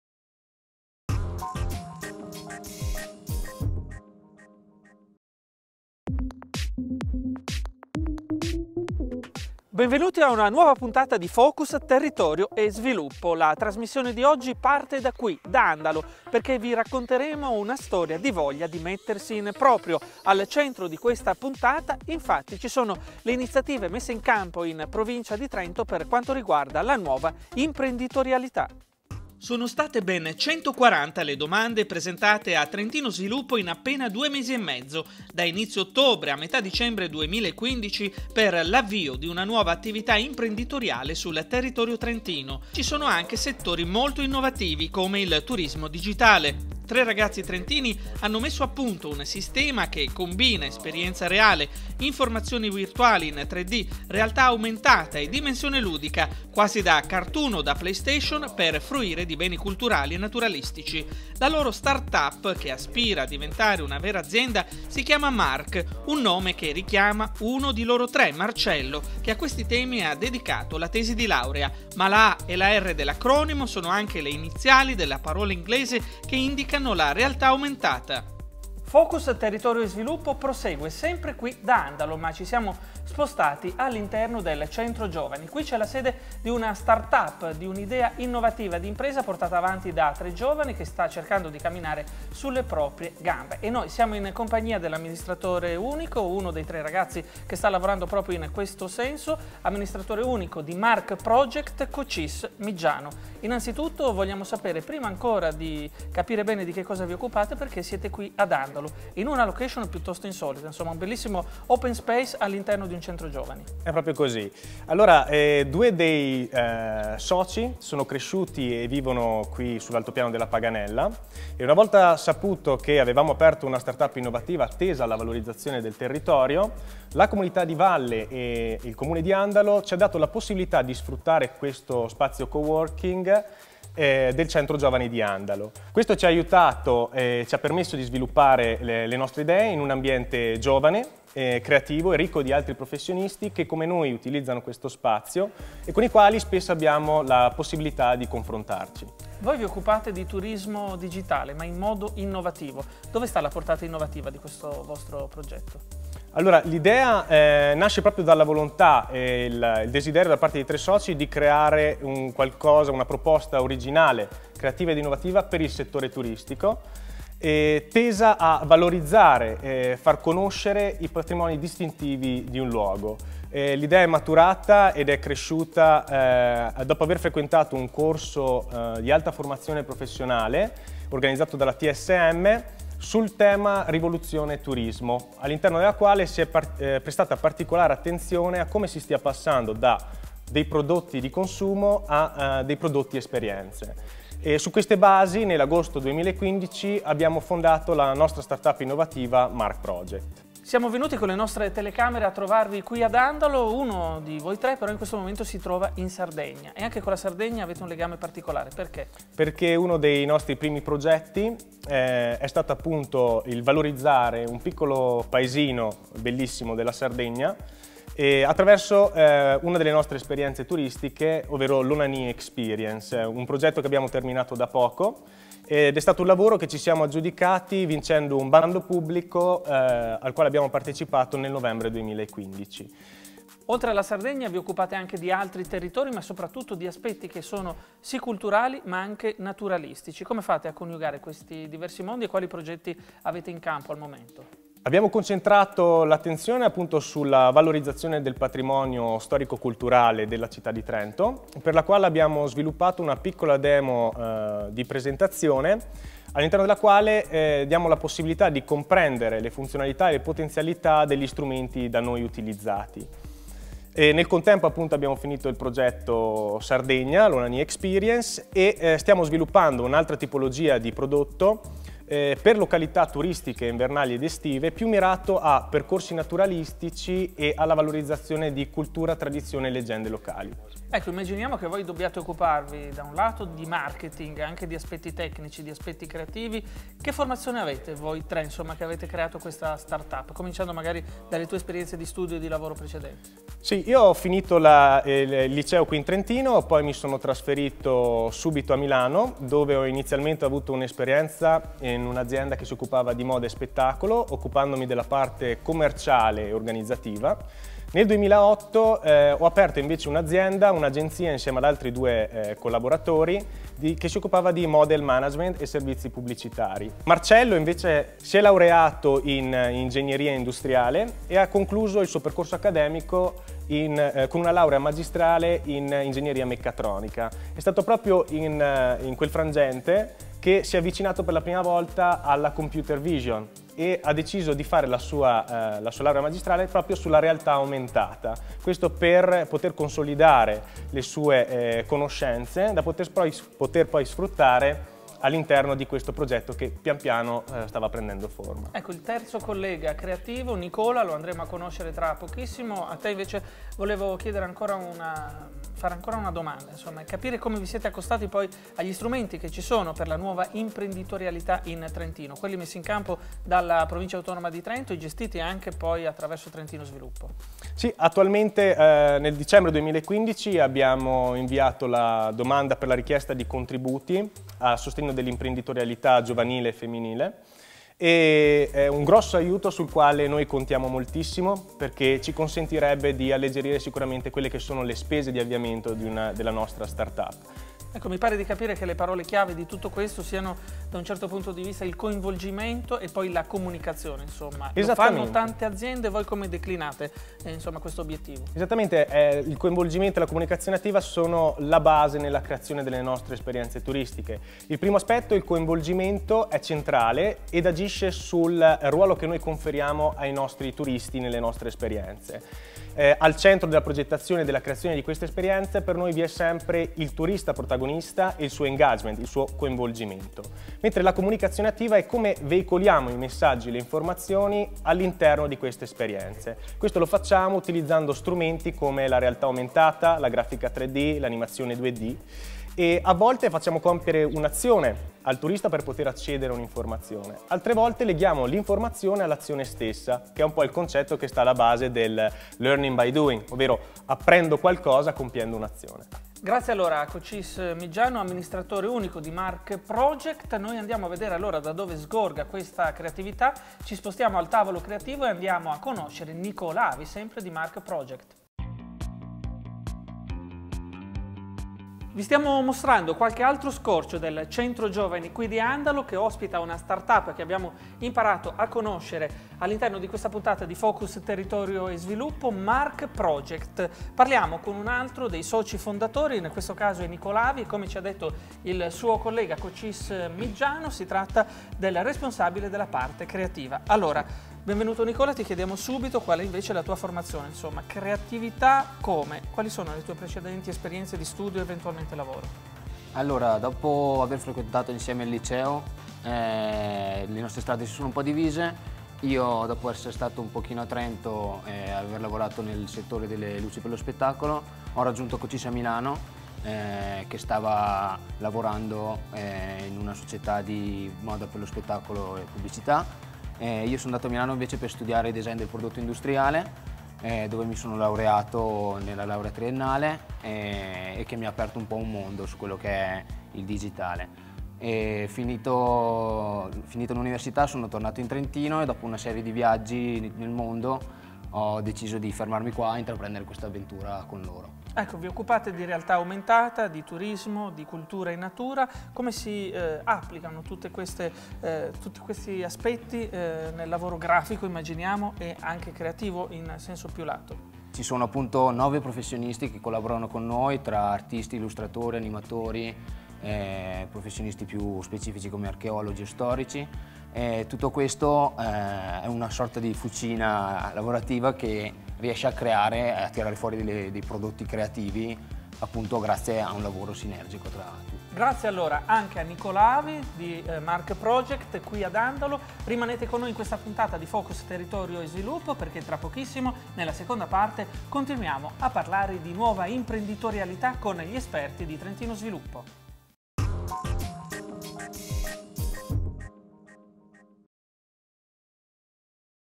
Intervista mARc project per Trentino TV
Non ha ancora compiuto un anno ma la startup mARc Project si è già fatta notare per innovazione e creatività, lavorando a Trento ed in Sardegna. L'idea? Rivoluzionare la comunicazione nel turismo, mischiando 3D, realtà aumentata e videogame con natura, esperienze, cultura e tradizione.